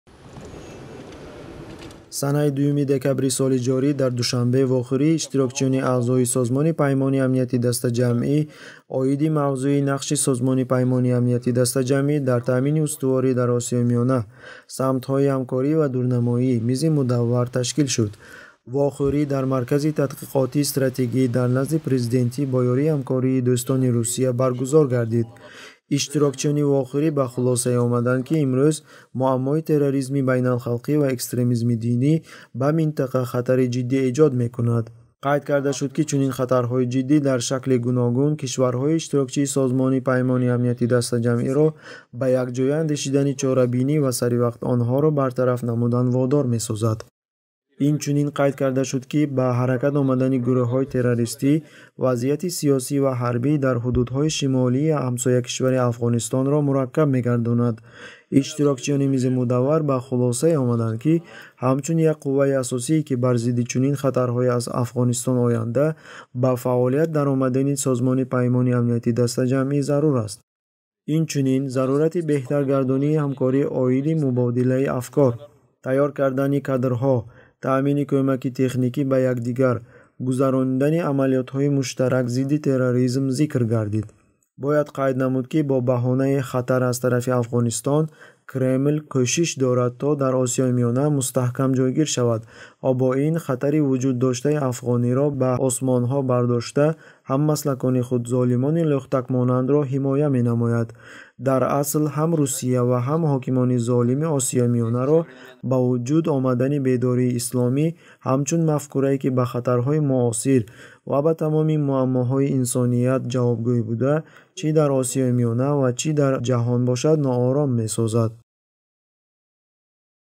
Русия ҳузури ҳарбии худро дар Тоҷикистон мустаҳкам мекунад
Санаи 2 декабри соли ҷорӣ дар Душанбе вохӯрии иштирокчиёни аъзои Созмони Паймони Амнияти Дастаҷамъӣ оиди мавзӯъи "Нақши СПАД дар таъмини устуворӣ дар Осиёи Миёна: самтҳои ҳамкорӣ ва дурнамоӣ" мизи мудаввар ташкил шуд. Вохӯрӣ дар маркази тадқиқотии стратегӣ дар назди президентӣ бо ёрии ҳамкории "Дӯстони Русия" баргузор гардид. Иштирокчиёни вохӯрӣ ба хулосае омаданд, ки имрӯз муаммои терроризми байналхалқӣ ва экстремизми динӣ ба минтақа хатари ҷиддӣ эҷод мекунад.